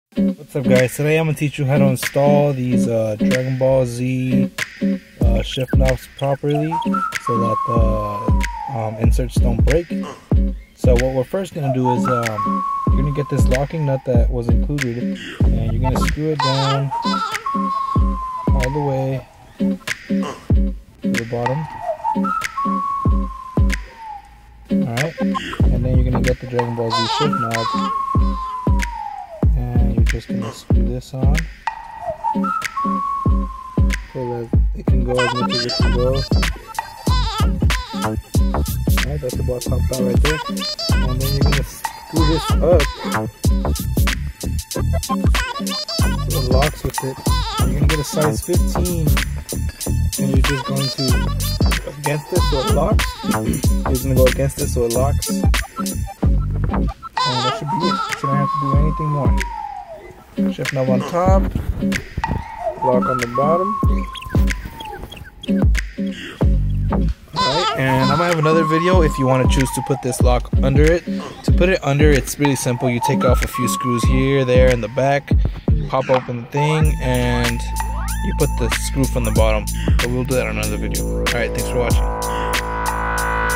Proper Shift Knob Install
This video is to describe the proper and safe way to install the shift knob.

You place the locking nut first and screw it all the way down, then place the shift knob and screw it all the way DO NOT OVER TIGHTEN.

then bring the lock nut up, and securely give it a small tug with a size 15 wrench. (may be different size)